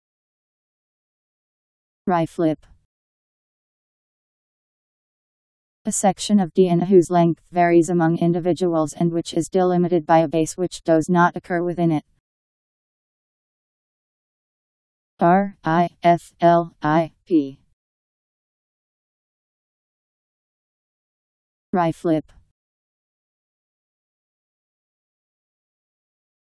Riflip Meaning
Video shows what riflip means. A section of DNA whose length varies among individuals and which is delimited by a base which does not occur within it..  Riflip Meaning. How to pronounce, definition audio dictionary. How to say riflip. Powered by MaryTTS, Wiktionary